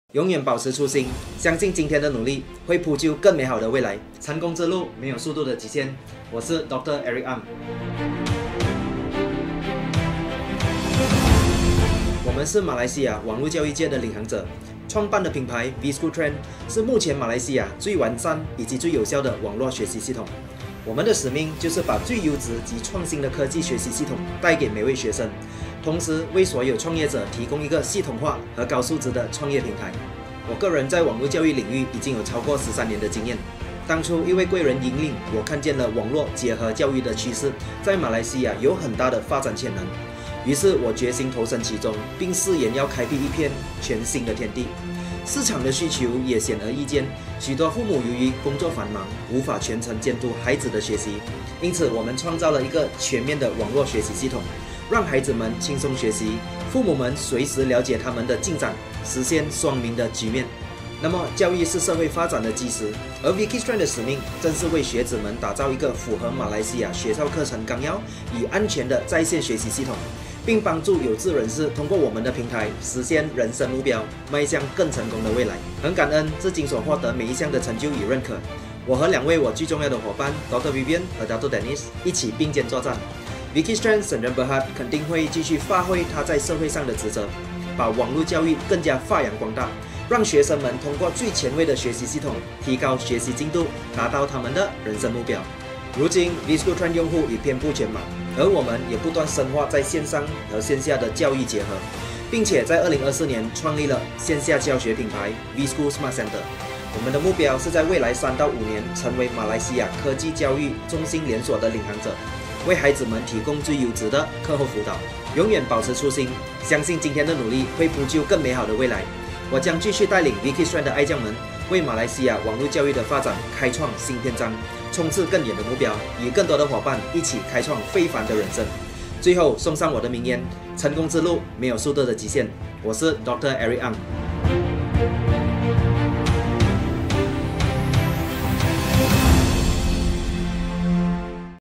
Working with Dr. Eric Ang - VKids Trend Co-Founder (100 Most Influential Young Entrepreneurs)
Always stay true to your original aspirations
Believe that today's efforts will pave the way for a better future
There are no speed limits on the road of success
- Dr. Eric Ang